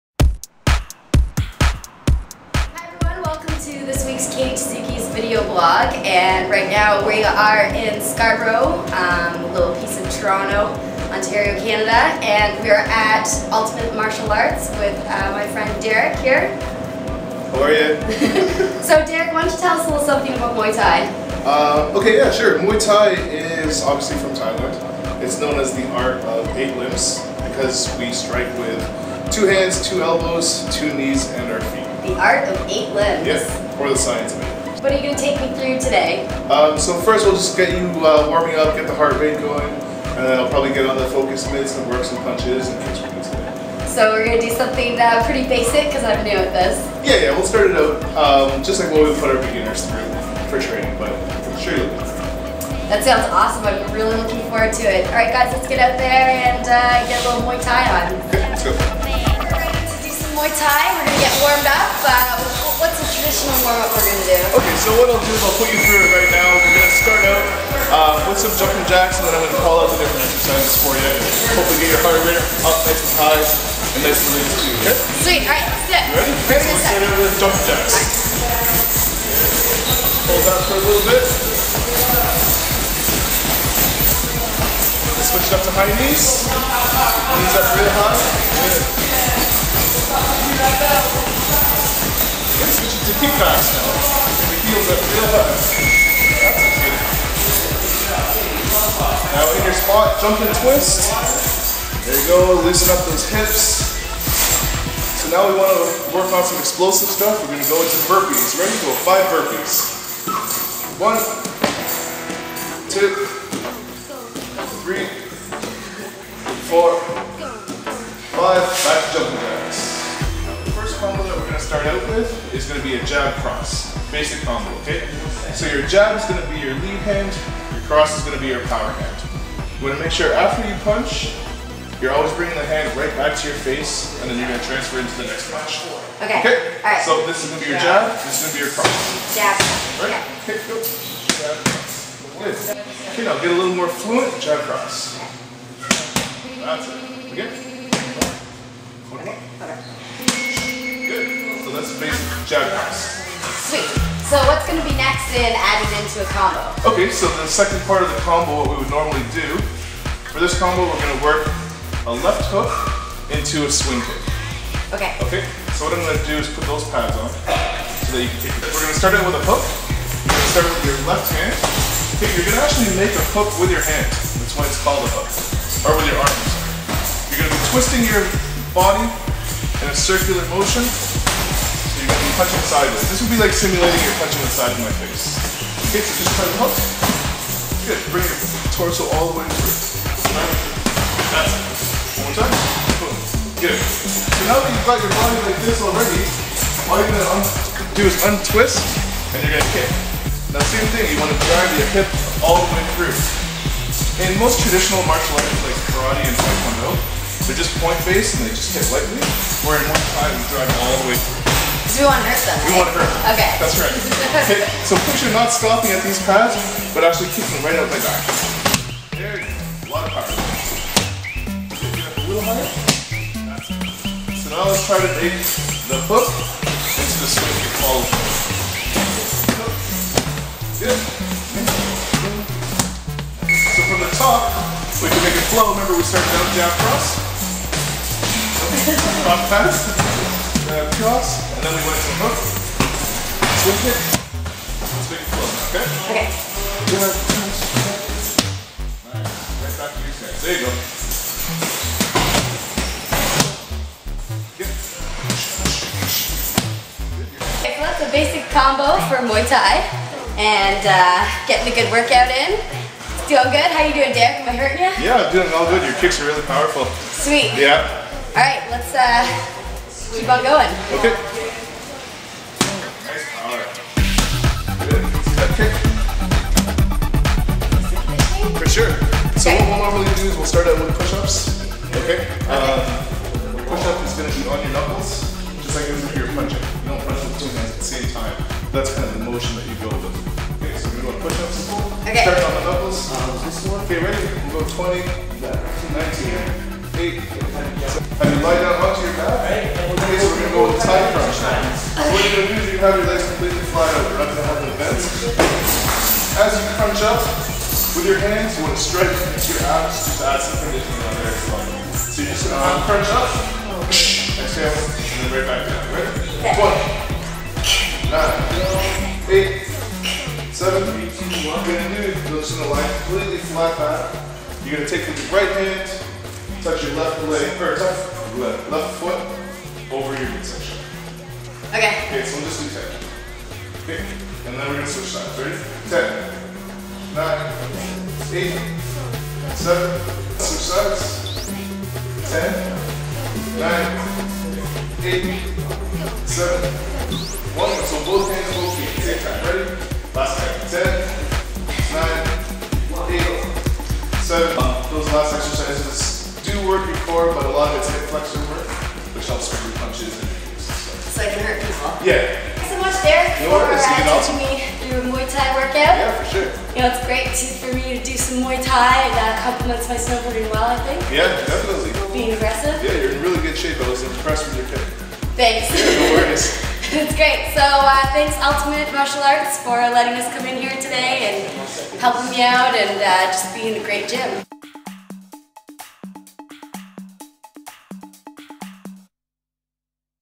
Katie Tsuyuki's Video Blog 8: Ultimate Martial Arts
Katie takes a Mauy Thai lesson.  Filmed with SONY Bloggie cameras.